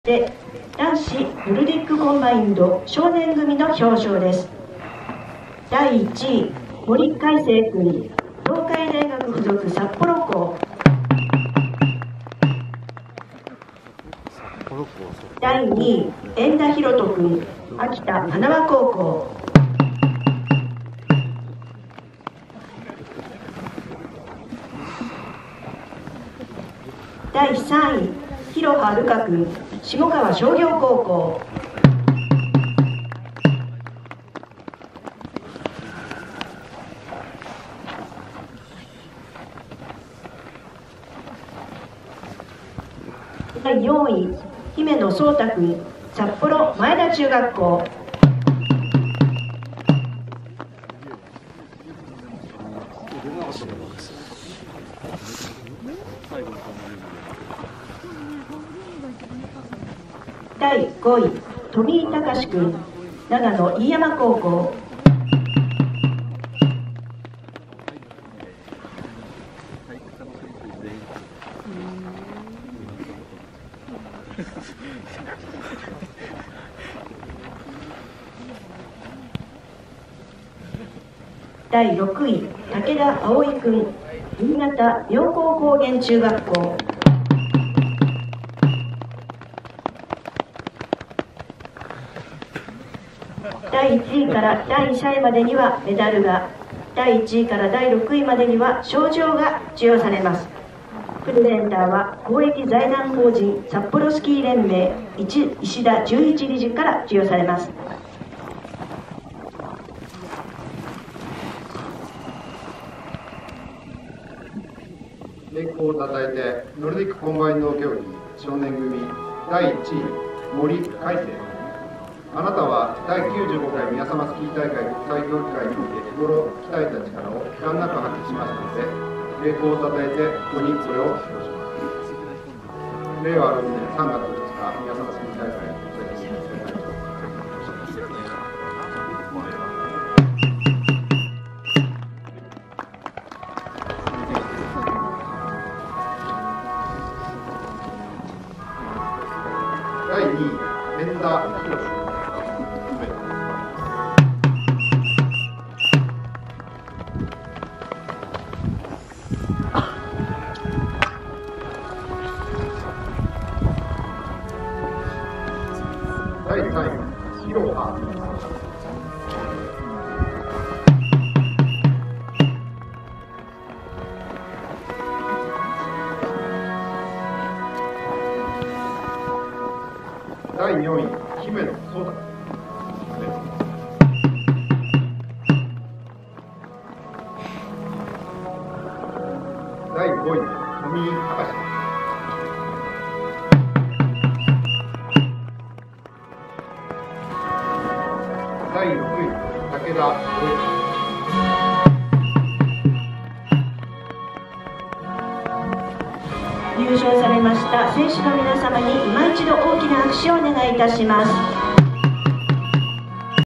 １位 森恢晟 選手、２位 遠田廣斗 選手、３位 廣波琉海 選手 第95回宮様コンバインド 表彰式 少年組 2024/03/02(土) 白旗山競技場
４位 姫野蒼大 選手
５位 富井孝 選手
６位 竹田葵 選手